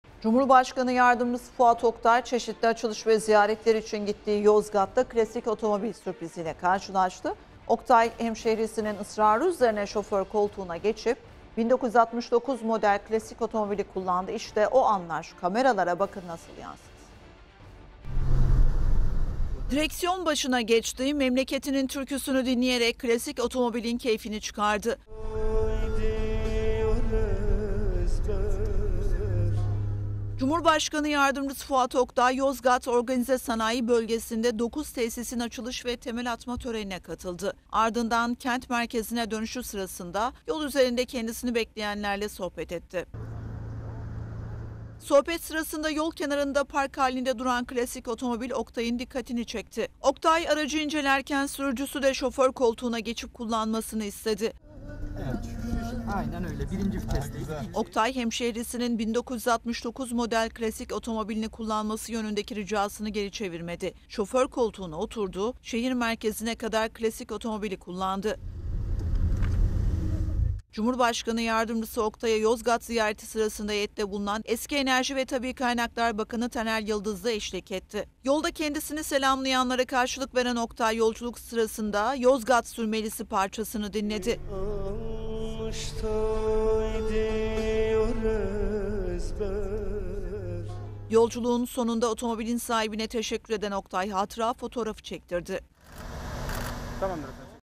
Fuat Oktay Klasik Otomobil Kullandı
Cumhurbaşkanı Yardımcısı Fuat Oktay, çeşitli açılış ve ziyaretler için gittiği Yozgat'ta klasik otomobil sürpriziyle karşılaştı. Oktay, hemşehrisinin ısrarı üzerine şoför koltuğuna geçip 1969 model klasik otomobili kullandı. İşte o anlar kameralara bakın nasıl yansıdı…